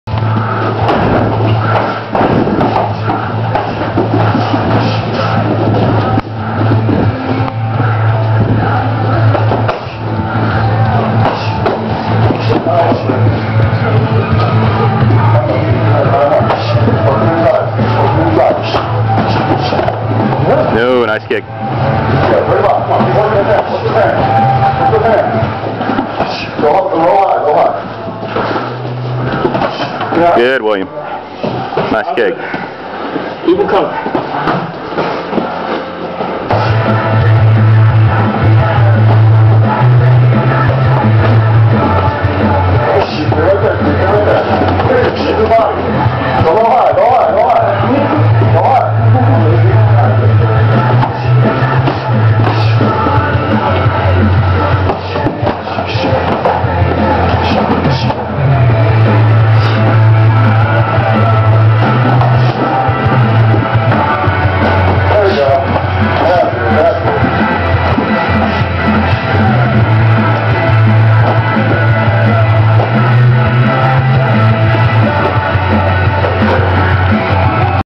William Badders Vs. Wayne Psencik
Sat morning sparring light contact Higher Power Martial Arts William Badder a 2nd degree black belt instructor Vs Wayne Psencik Brown belt senior student.  Hutto TX  MMA